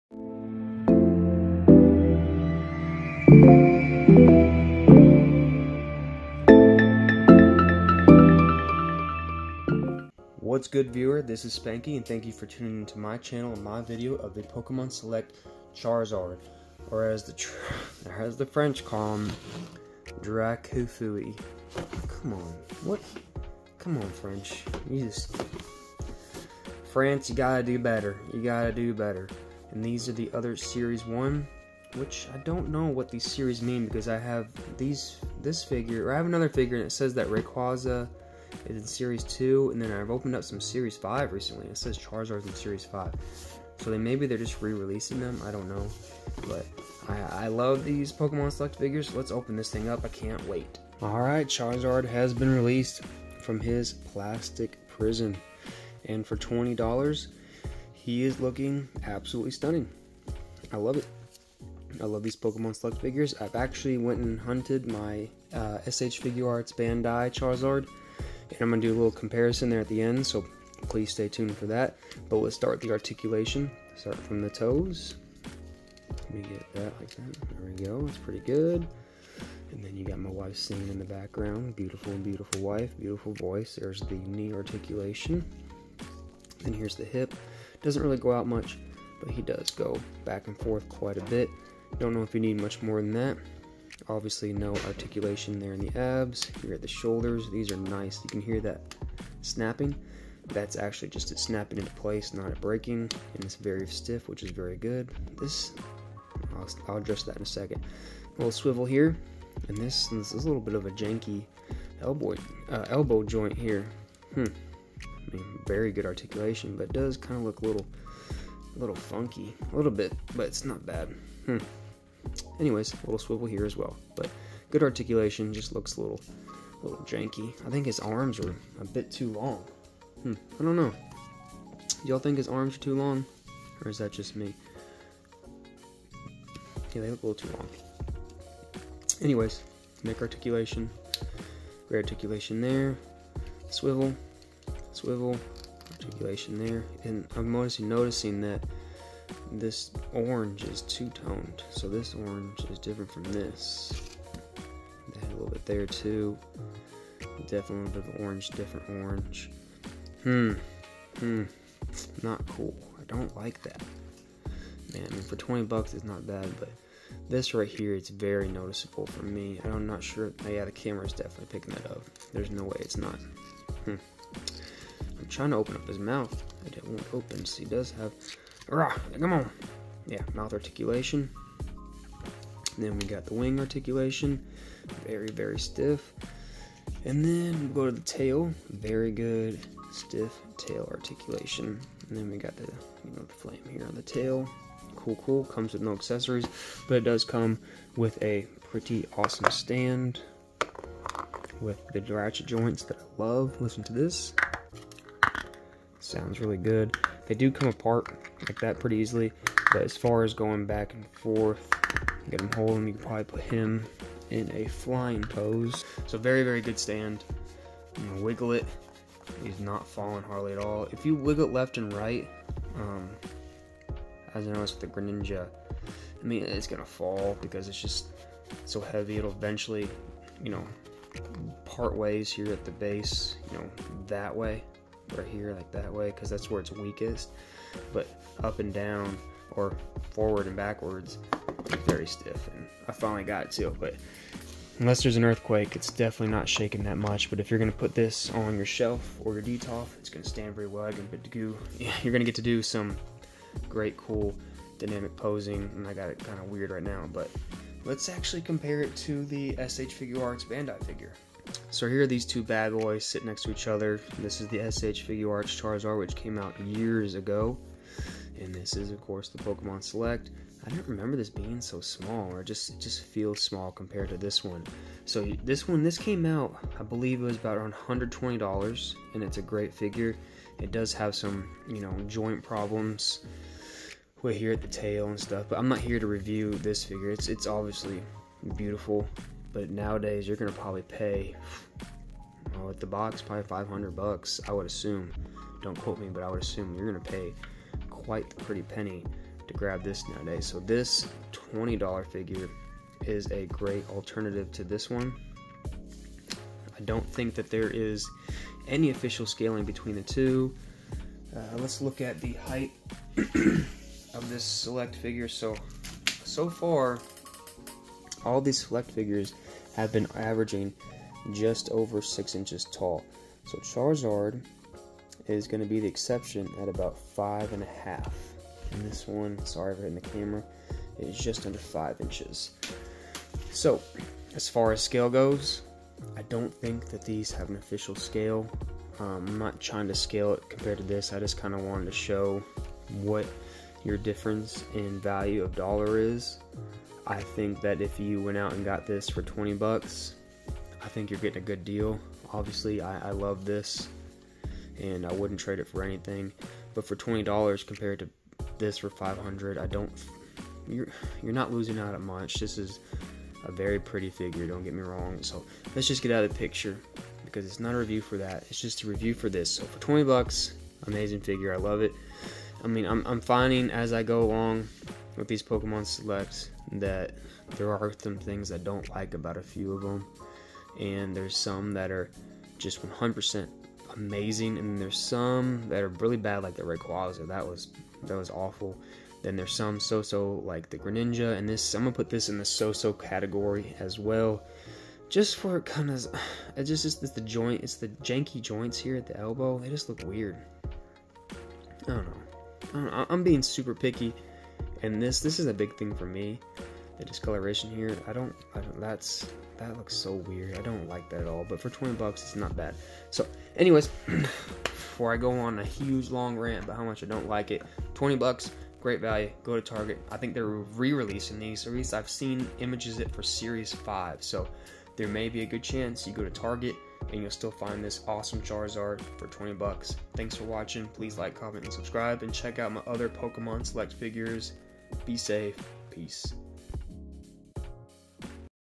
Charizard Select Pokemon Super Articulated 6-Inch Figure
15 plus Points of Articulation.
Incredible detail that makes this First Partner Pokémon look like it jumped from the Pokémon animated series.
Comes with a Posing Arm accessory for maximum display potential.
Collect all of Wave 1 of the Select 6 Super Articulated figures: Articuno, Greninja, and Charizard
An officially licensed Pokémon product from Jazwares.